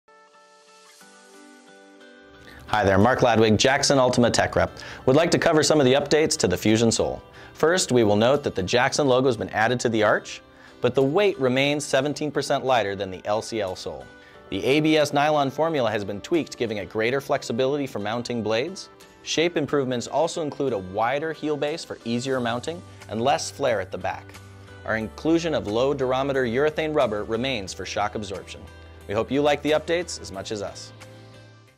Jackson Ultima Fusion Sole for Retailers
The Fusion Sole is constructed from lightweight nylon composite which is torque resistant and waterproof.  Rubber inlay provides non-slip blade mount and superior shock absorption.